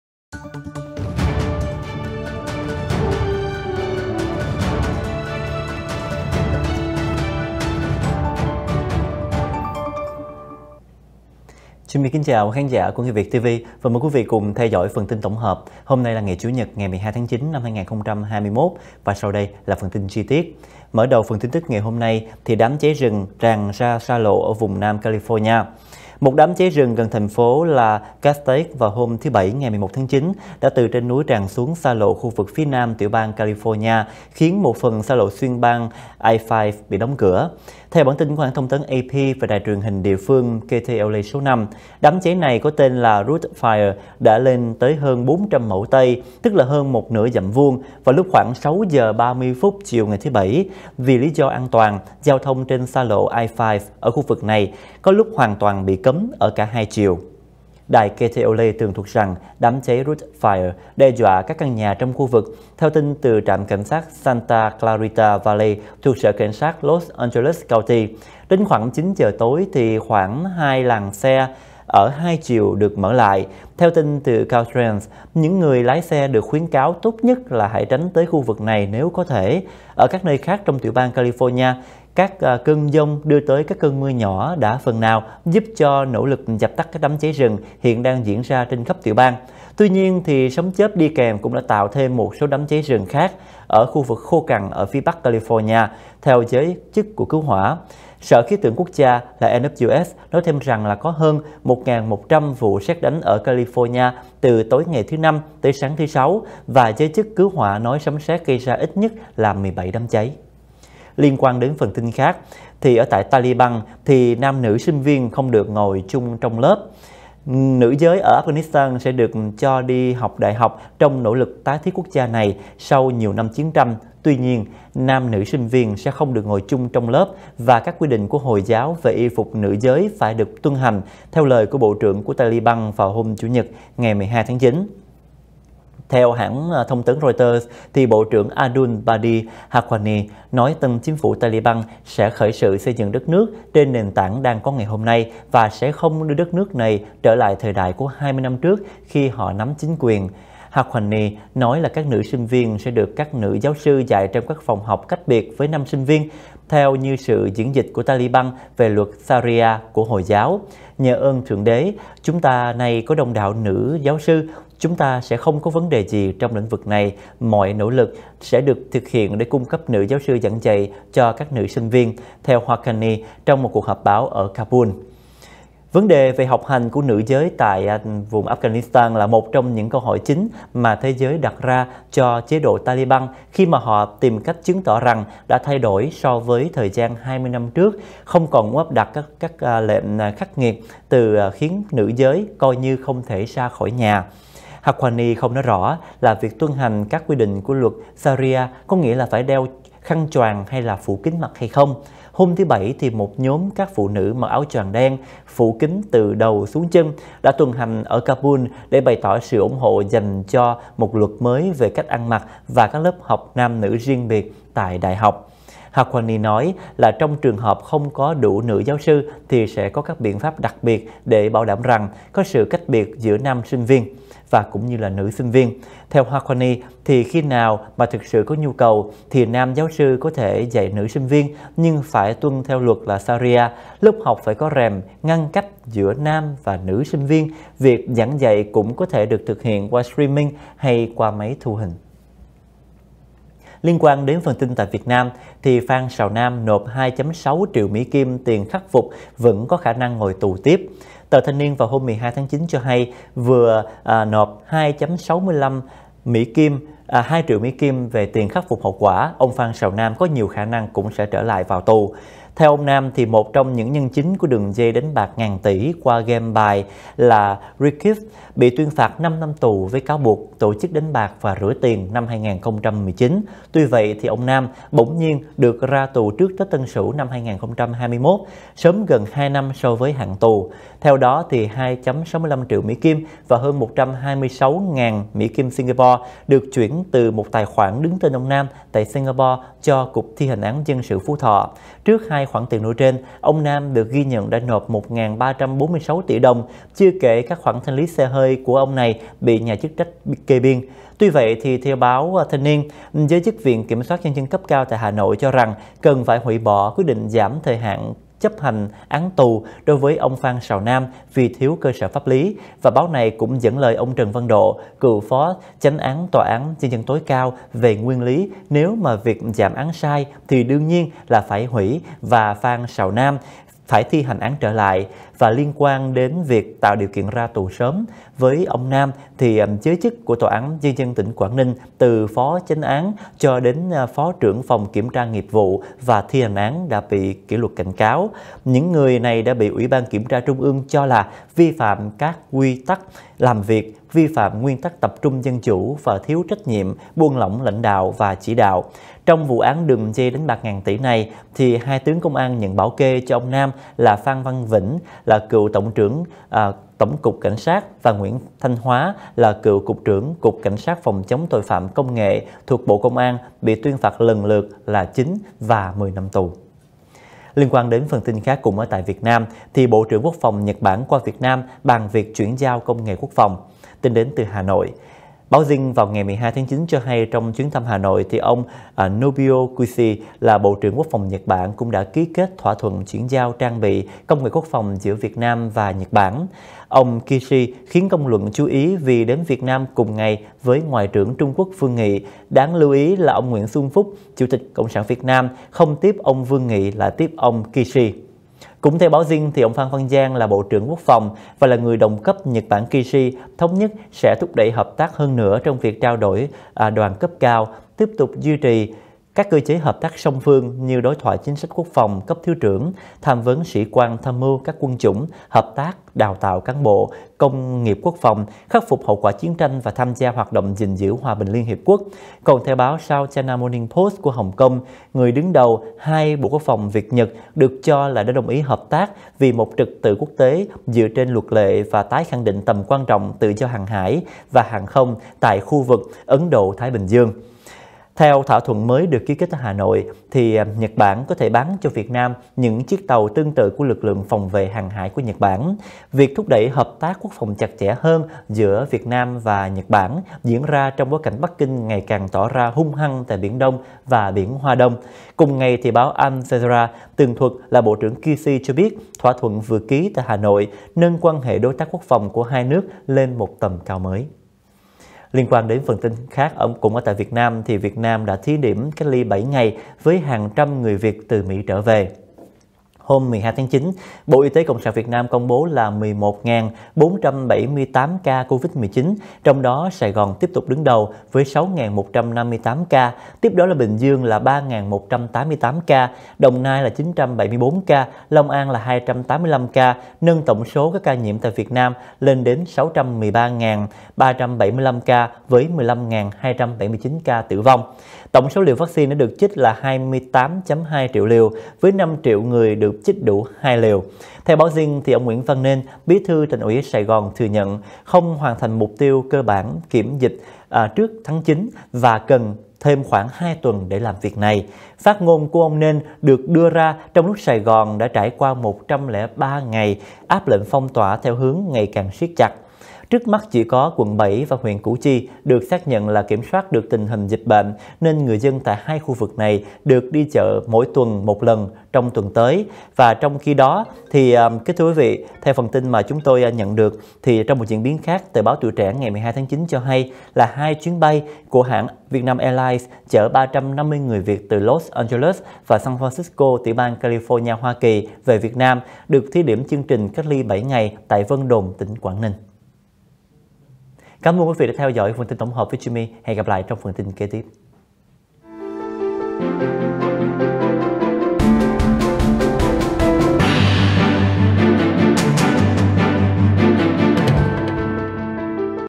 Hàng trăm người Việt từ Mỹ về VN được thí điểm ‘cách ly 7 ngày ‘
-Đám cháy rừng tràn ra xa lộ ở vùng Nam California
-Taliban: nam nữ sinh viên không được ngồi chung trong lớp
-Phan Sào Nam nộp $2.6 triệu ‘tiền khắc phục’ vẫn có khả năng ngồi tù tiếp
-Bộ trưởng Quốc Phòng Nhật qua Việt Nam bàn việc ‘chuyển giao công nghệ quốc phòng’